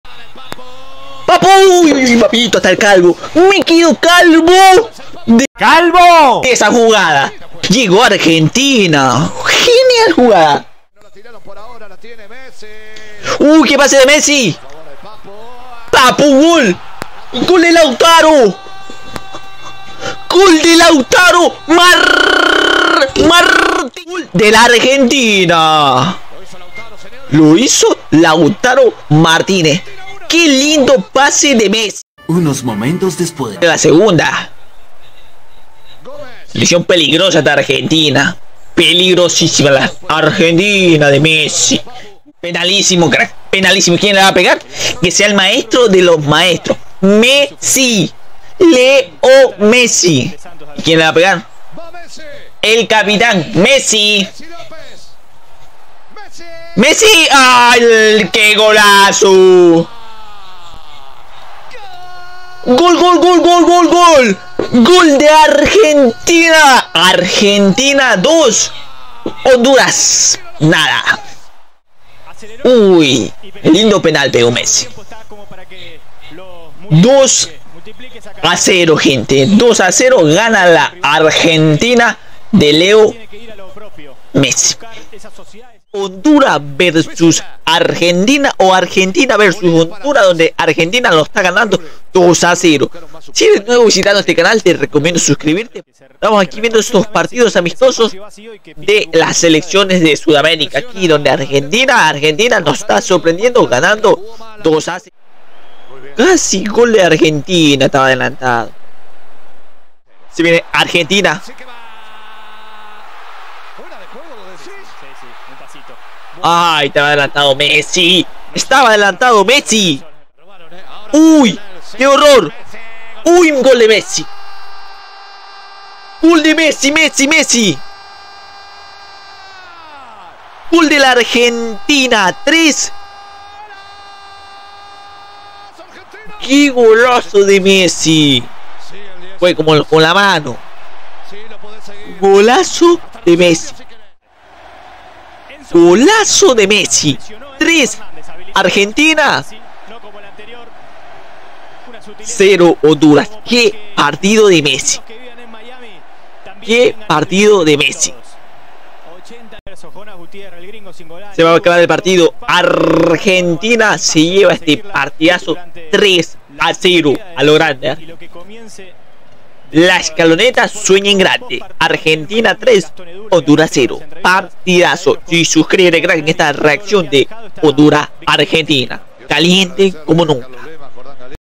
ARGENTINA vs HONDURAS (3-0) REACCIÓN DE HINCHA ARGENTINA
reaccionando a argentina vs Honduras (3-0)
Amistoso internacional